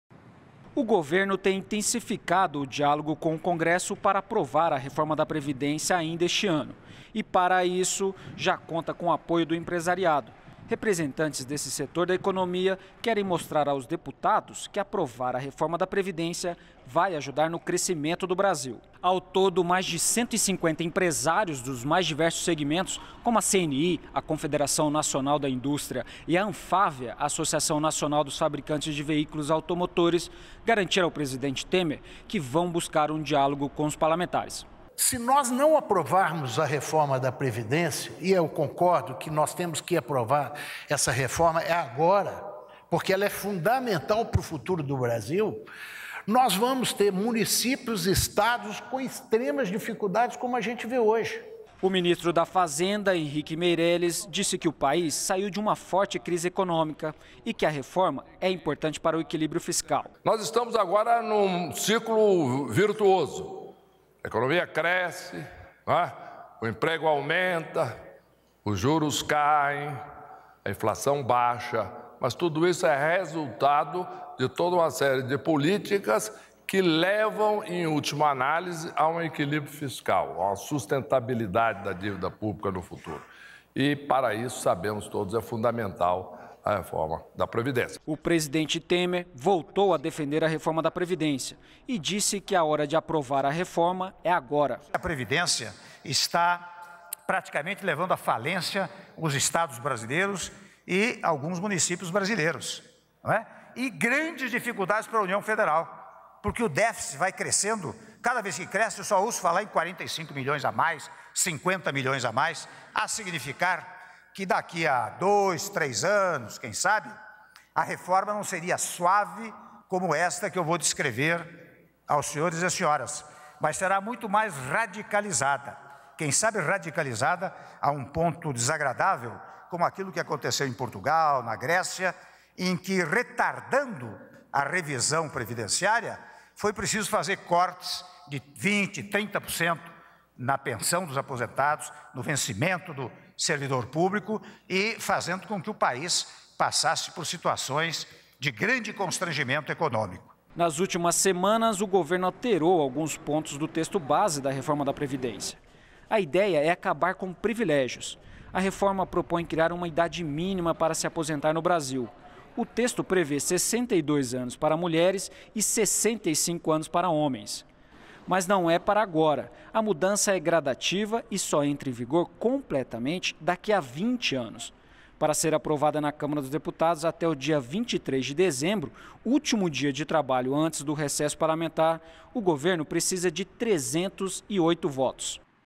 Presidente Temer se reúne com lideranças empresariais para discutir a reforma da Previdência
REPÓRTER NBR - 13.12.17: presidente Michel Temer se reuniu, nesta terça-feira (12), com lideranças empresariais. Na reunião o presidente voltou a defender a necessidade de aprovação da reforma da Previdência.